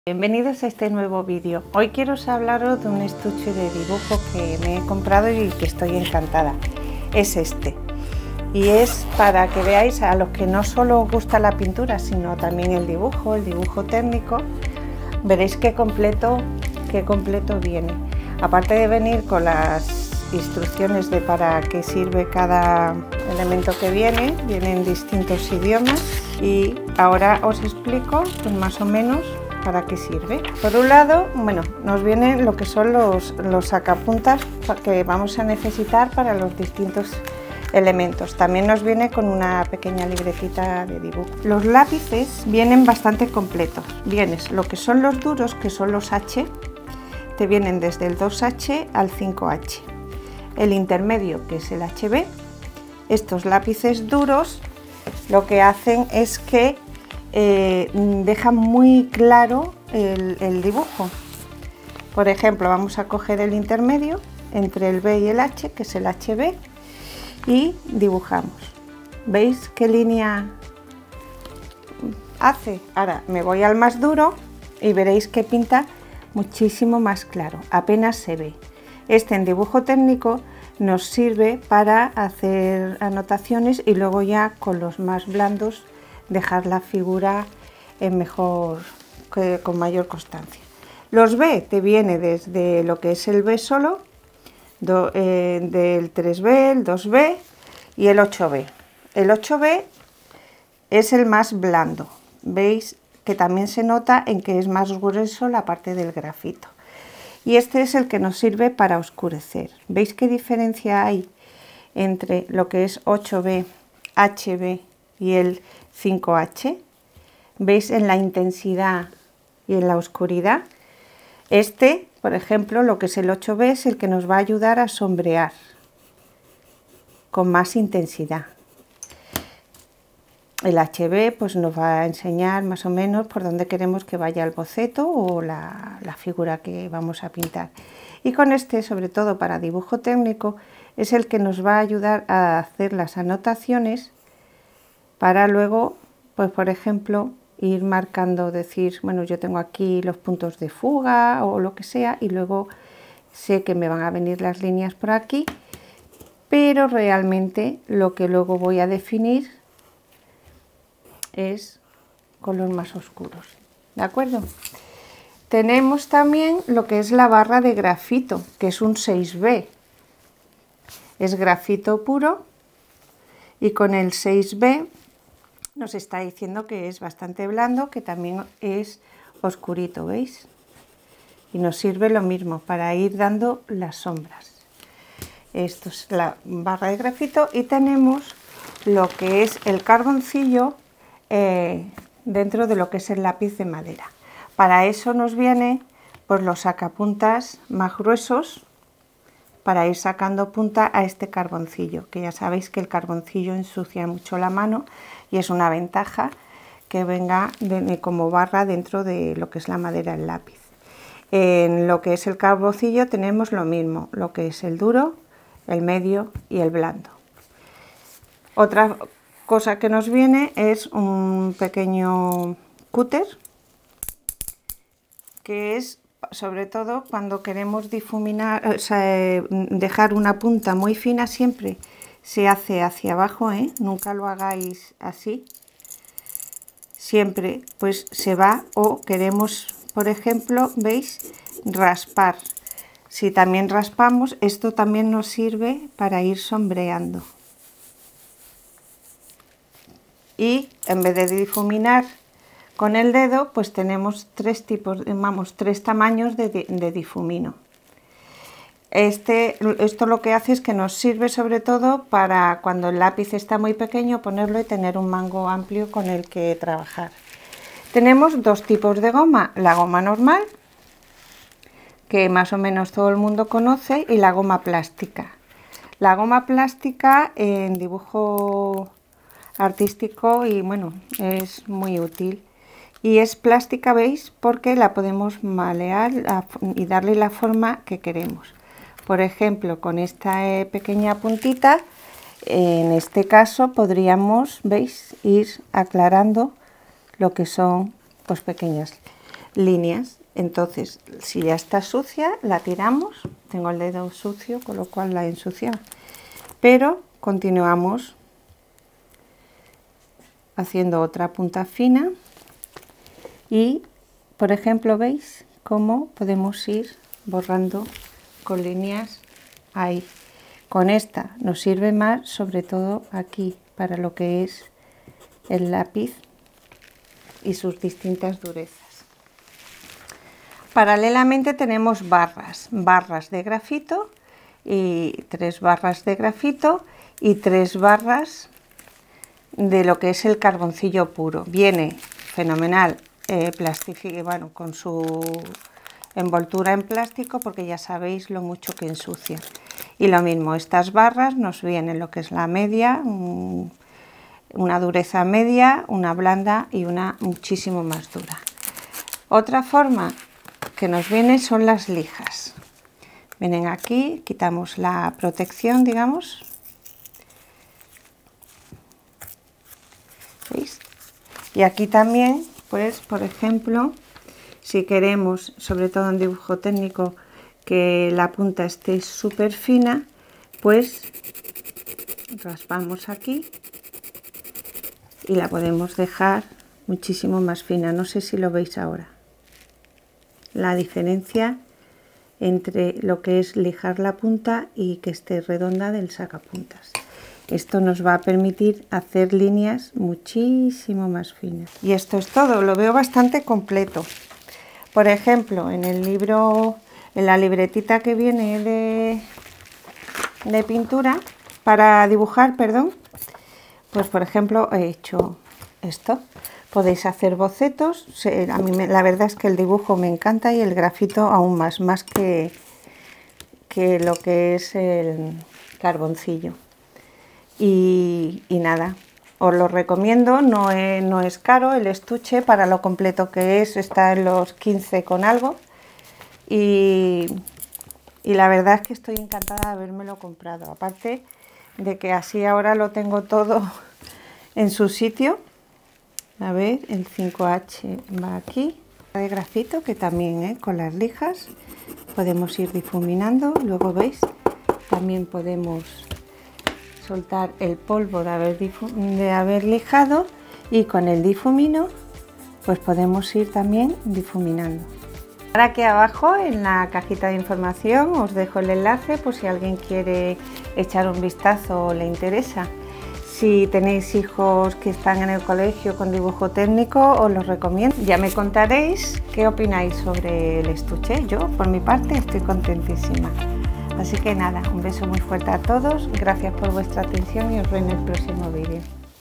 Estuche Lapices para bosquejo, dibujo artístico y técnico
Review de un estuche básico de lapices para dibujo técnico y artístico. Por si os interesa enlace  :
Enlace a Amazon (España)